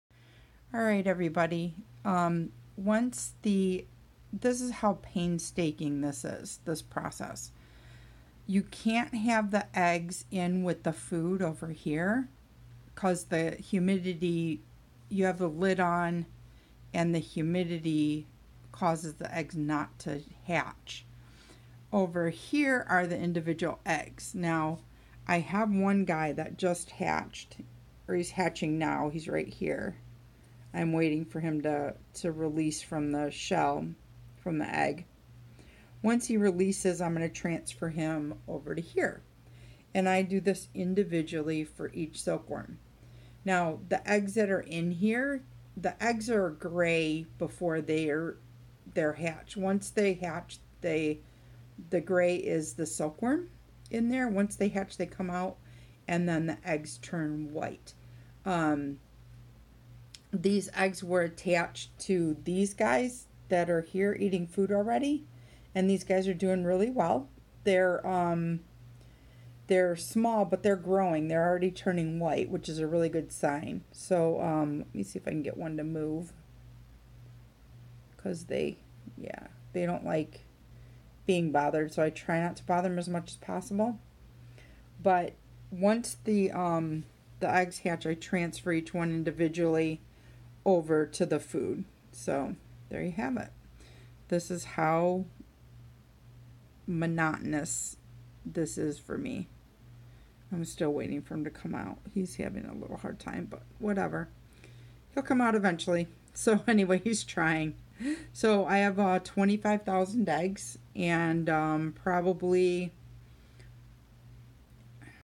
Silkworm Hatching Success
This is my painstaking process of hatching 25 thousand silkworm eggs.
I transfer one hatchling at a time, from egg holding Petrie dish (which I keep in an incubator) to the food Petrie dish.